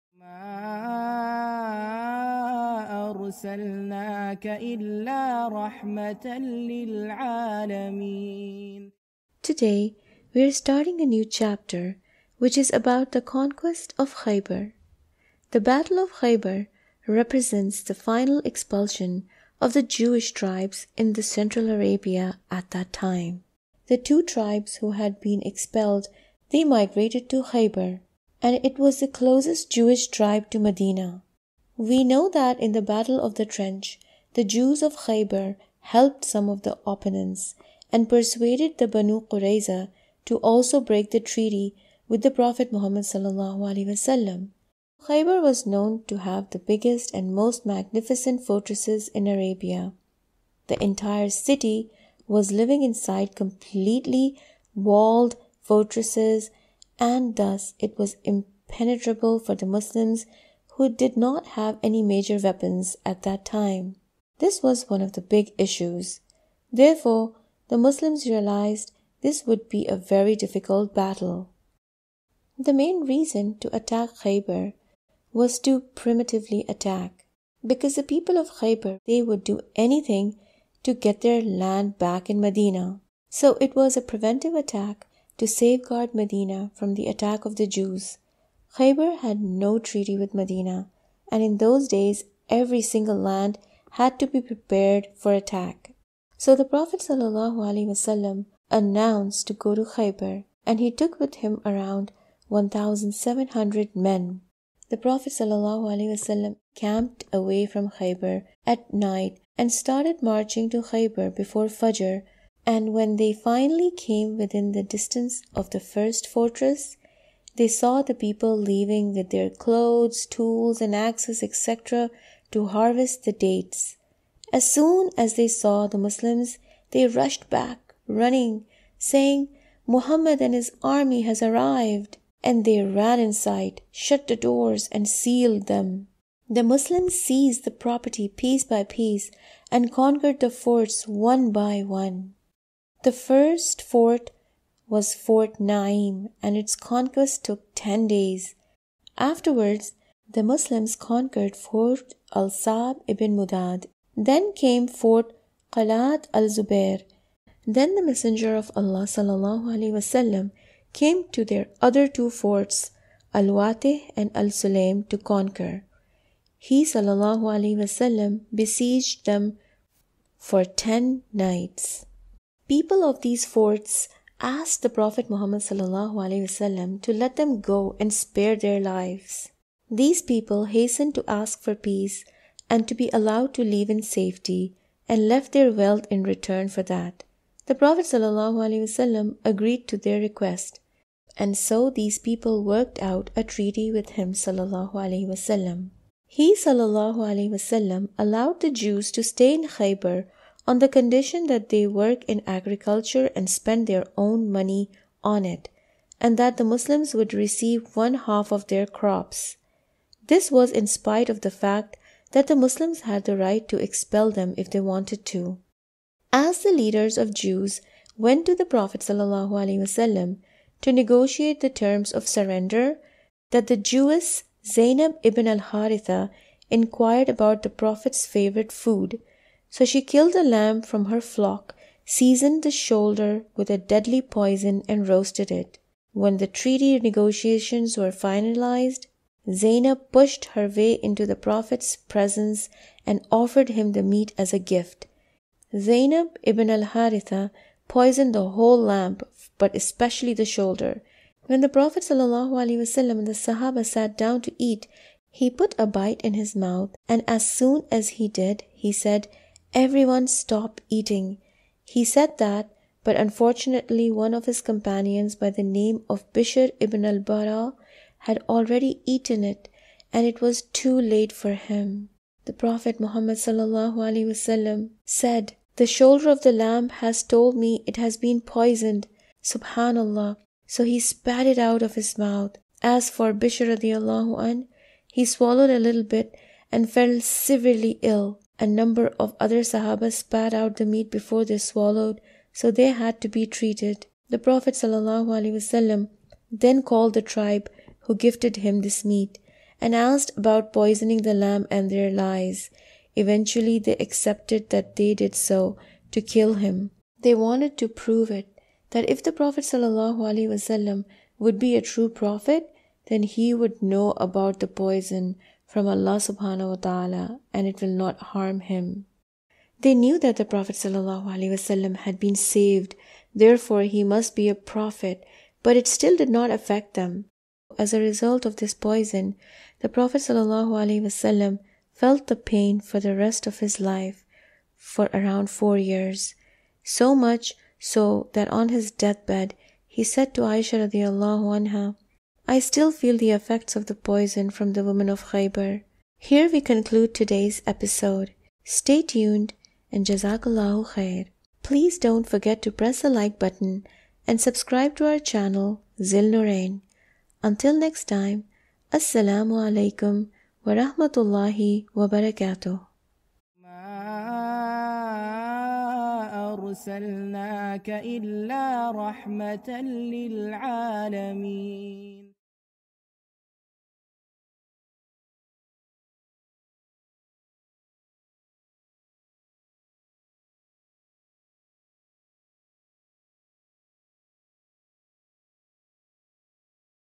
Events of Khaybar | Episode 67 | Seerah Series | Zill Noorain
The Muslims set out for Khaybar in March 628, Muharram AH 7. According to different sources, the strength of the Muslim army varied from 1,400 to 1,800 men and between 100 and 200 horses. Some Muslim women (including Umm Salama) also joined the army, in order to take care of the wounded.

Seerah Series for Kids, Episode 67, Events of Khaybar
Main channel @Zill Noorain
Our Quran Memorization channel @Zill Noorain Quran Memorization
Urdu channel @Zill Noorain / Urdu